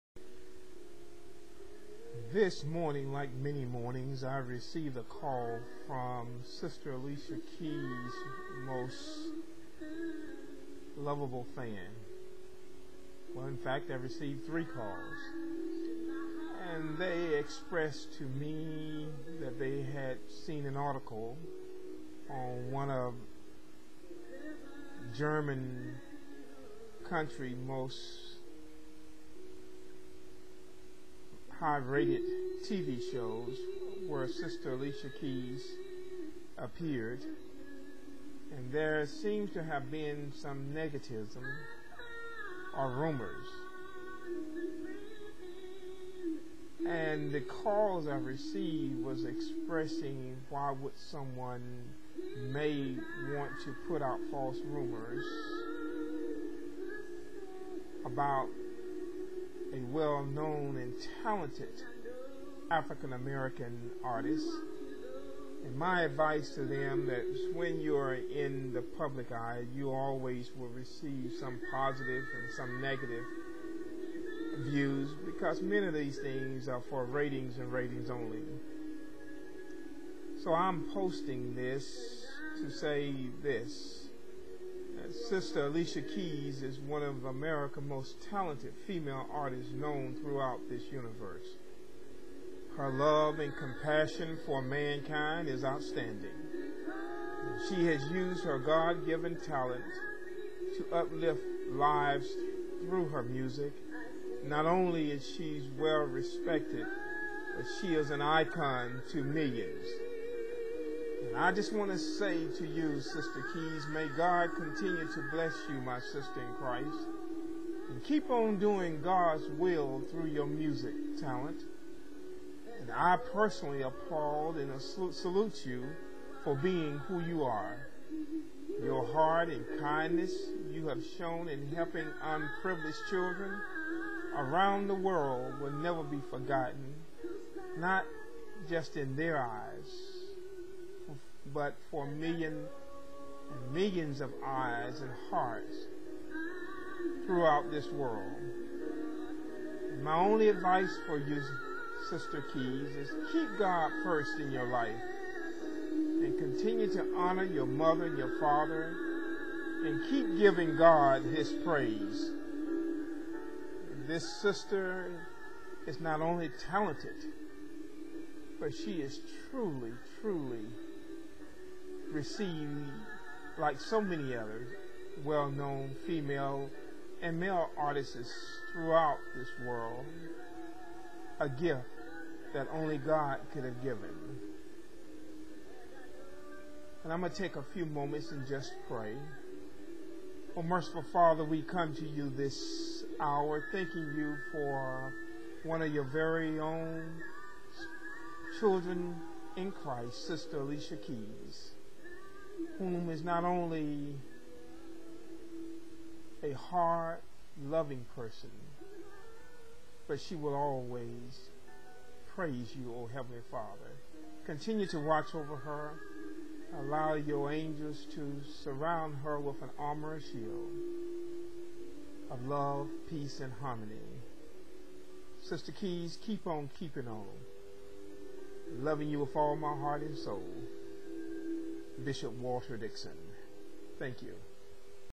Bishop Walter Dixon's Salute Music Icon Alicia Keys.wmv
Bishop Walter Dixon's Commentary Regarding Remarks On A German T.V. Show About Super Star and Icon Alicia Keys.